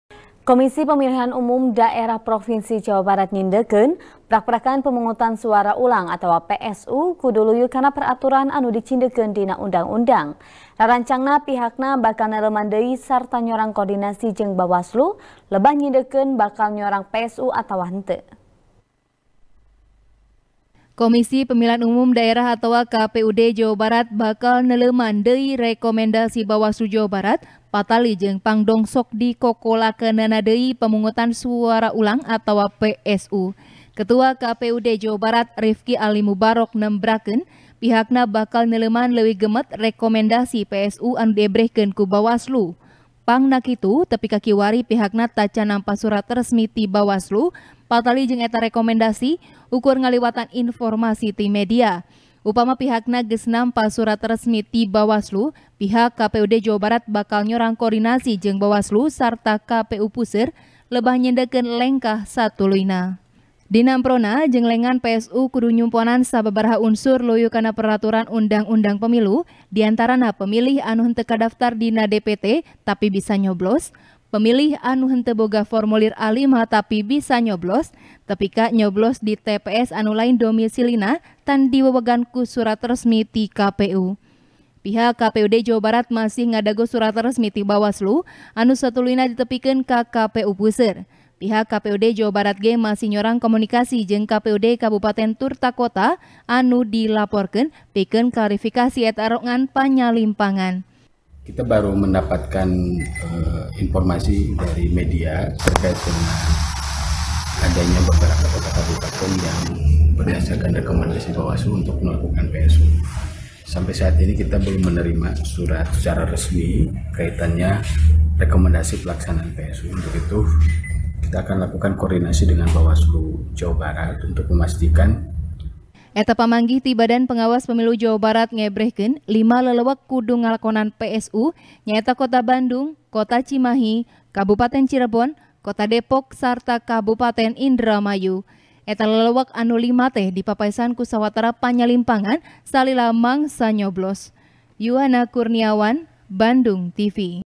KPUD JABAR Neuleuman Rarancang Nyoblos Ulang Di Sawatara TPS | TP | BANDUNGTV
TP | BANDUNGTV | ASTRI CAHYANI |Komisi Pemilihan Umum Daerah Provinsi Jawa Barat Nyindekeun, Prakprakan Pemungutan Suara Ulang Atawa P-S-U, Kudu Luyu Kana Peraturan Anu Dicindekeun Dina Undang-Undang. Rarancangna, Pihakna Bakal Neuleuman Deui Sarta Nyorang Kordinasi Jeung Bawaslu, Lebah Nyindekeun Bakal Nyorang P-S-U Atawa Henteu.